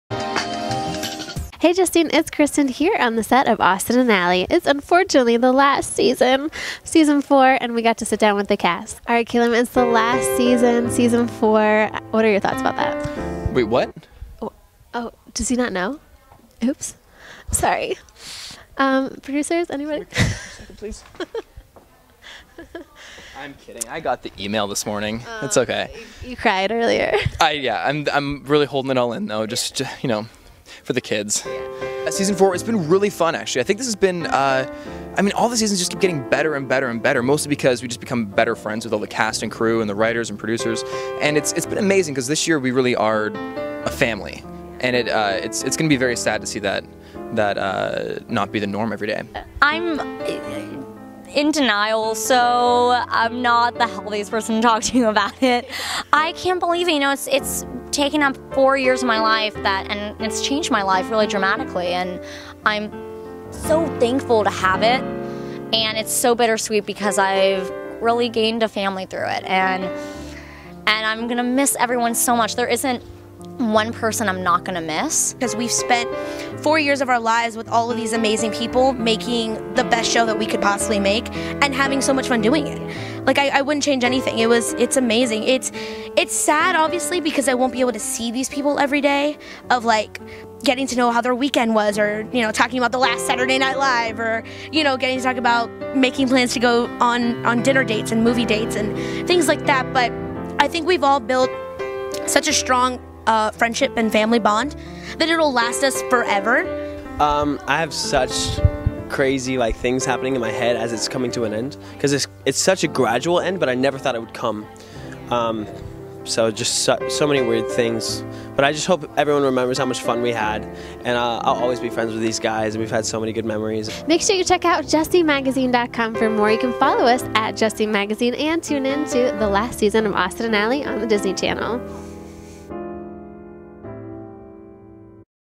Justine Magazine: Cast of "Austin & Ally" on Final Season 4!
Laura Marano, Ross Lynch, Raini Rodriguez and Calum Worthy share what they think about their final Season 4!
and follow us @justinemagazine!